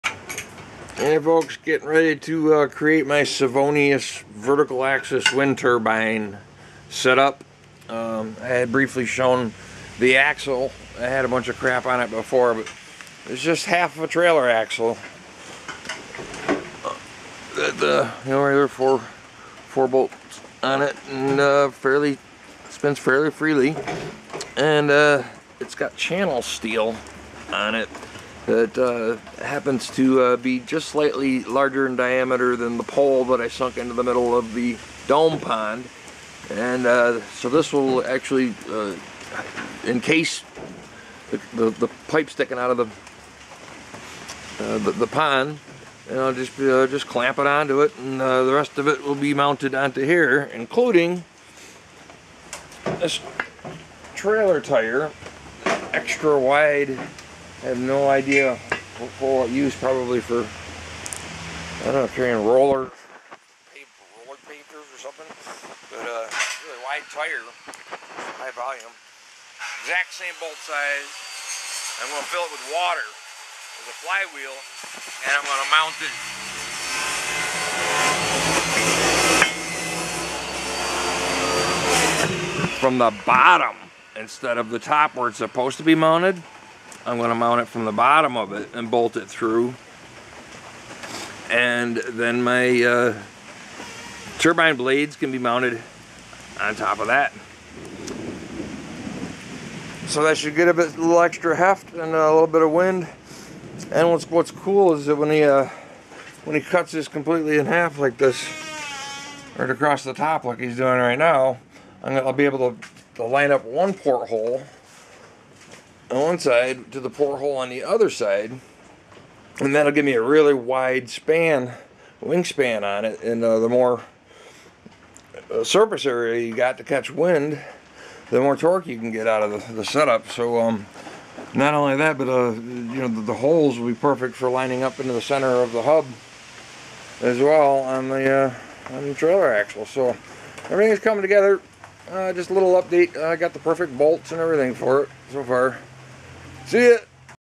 Savonius Vertical Axis Wind Turbine Part 1
This is a project I've been planning to build for a couple of years now and am thrilled to actually acheive some progress on it. Ideally, I'd like to use it to power a big two-cylinder air compressor head which is where the "fluidic flywheel" should come in handy (fingers crossed). I'll show the pond progress in another video.